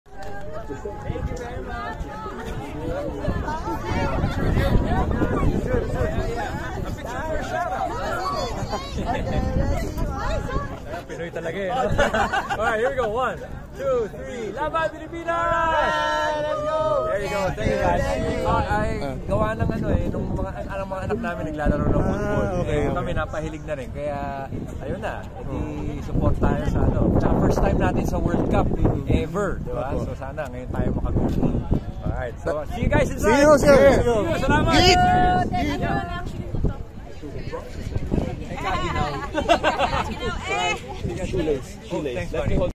Ryan Agoncillo and Judy Ann Santos support the Philippine team in Women's World Cup
Ryan Agoncillo and Judy Ann Santos, parents of budding football players, in Wellington to support the Philippine team in Women's World Cup.

Viber: Spin_ph